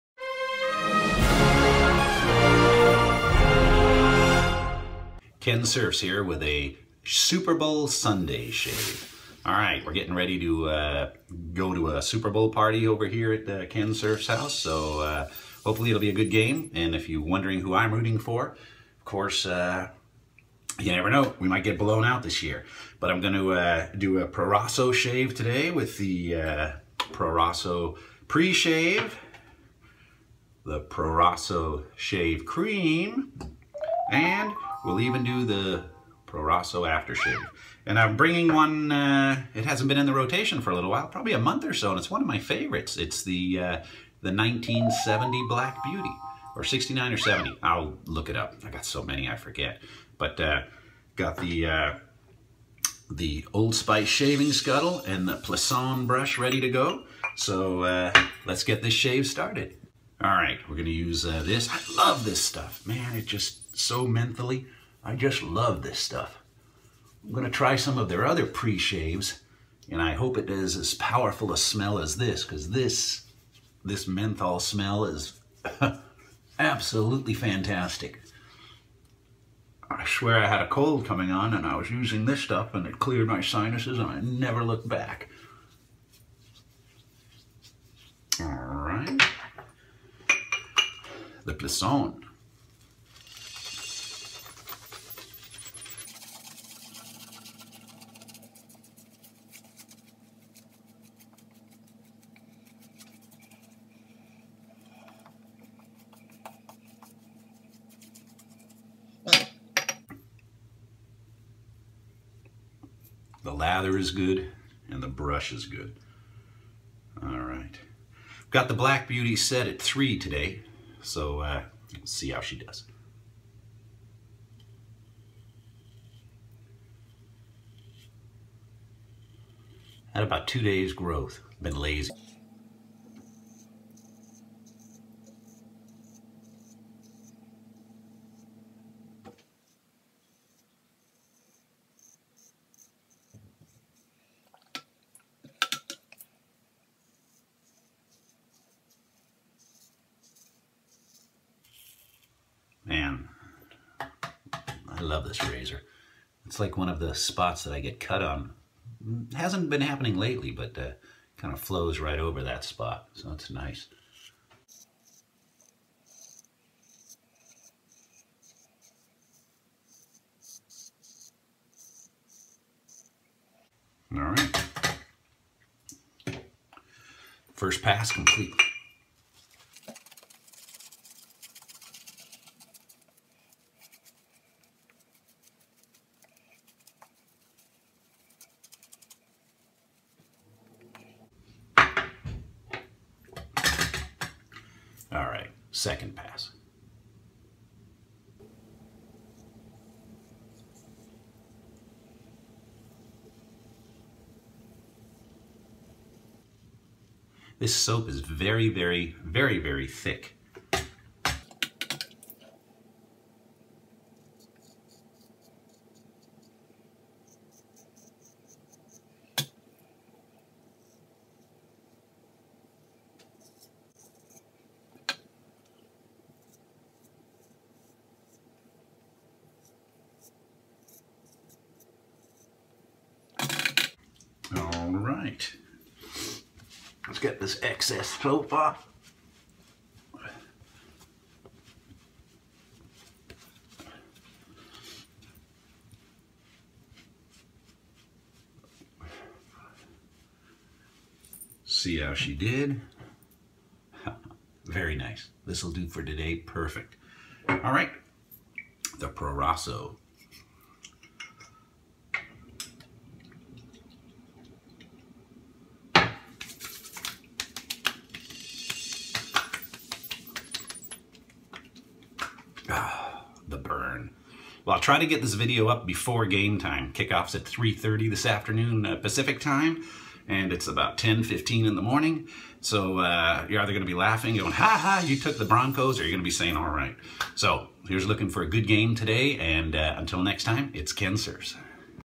Super Bowl Sunday Shave. With Gillette Black Beauty vintage razor.
A Super Bowl Sunday shave, with 1969 Gillette Black Beauty. Hoping that the Broncos win, but might be a Carolina blowout.

Razor - 1969 Gillette Black Beauty

Blade - Astra

PreShave - Proraso Menthol

Shave Soap - Proraso Creme

Aftershave - Proraso Green